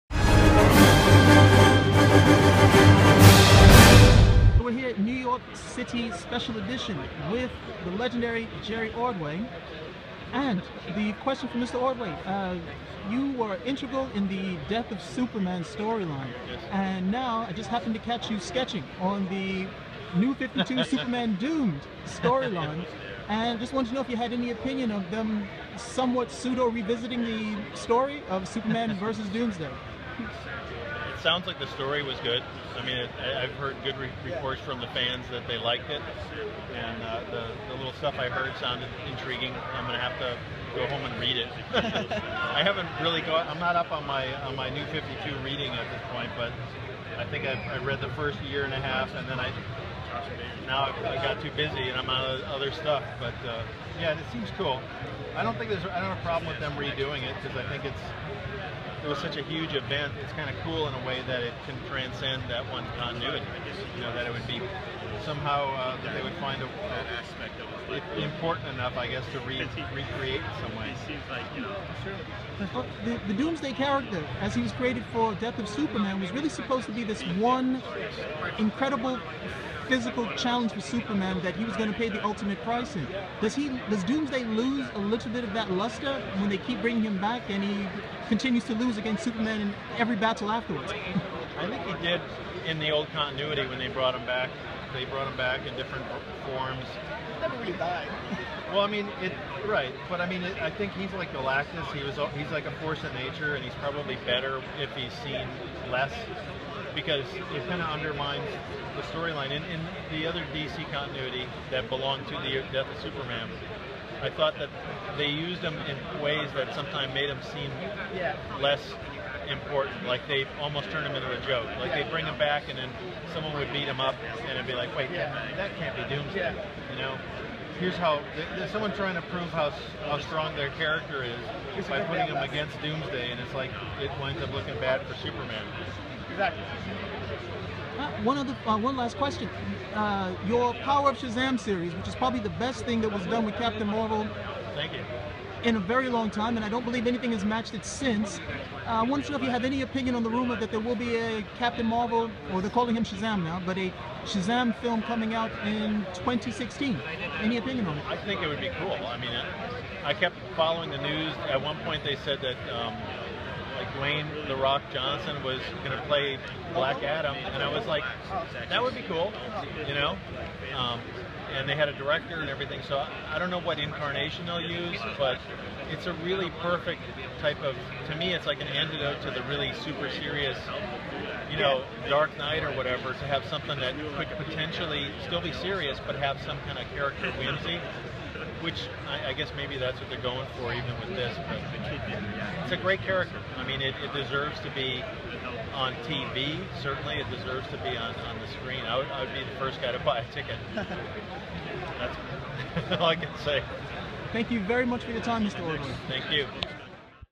Exclusive Jerry Ordway Video Interview - Superman and Doomsday
Superman Homepage writer T.A. Ewart caught up with comic book creator Jerry Ordway at this past weekend's Special Edition: New York City convention held at the Javits Center North, NYC, and asked him for his thoughts on the "Superman: Doomed" saga currently taking place in the Superman comic books.